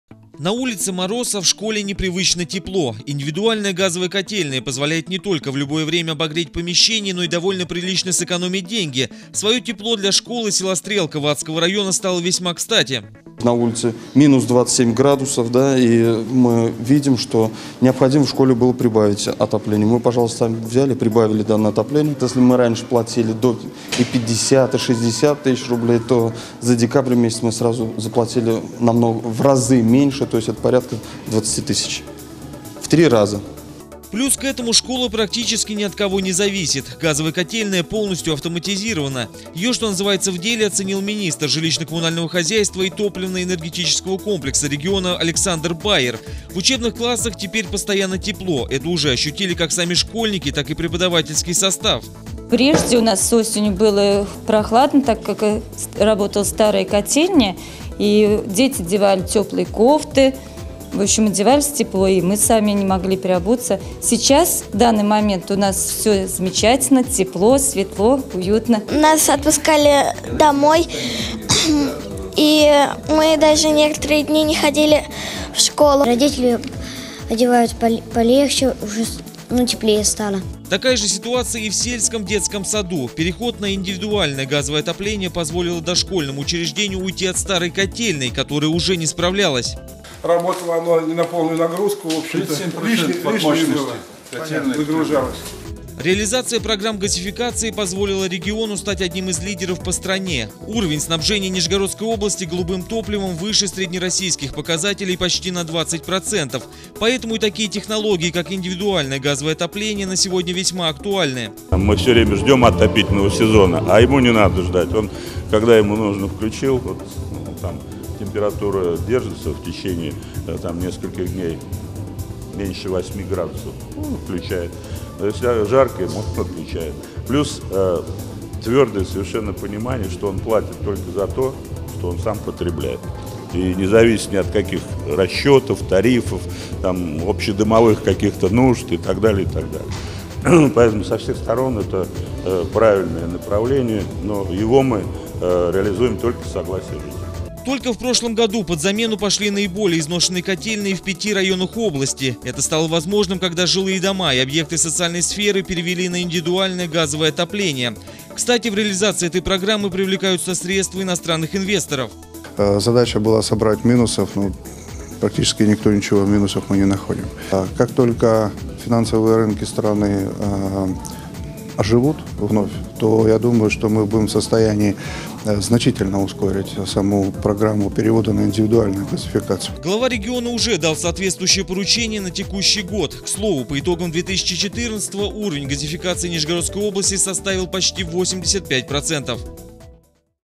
Дошкольные учреждения Вадского района перевели на газовую систему оборудования
На улице мороз, а в школе непривычно тепло. Индивидуальная газовая котельная позволяет не только в любое время обогревать помещение, но и довольно прилично сэкономить деньги. Своё тепло для школы села Стрелка Вадского района стало весьма кстати.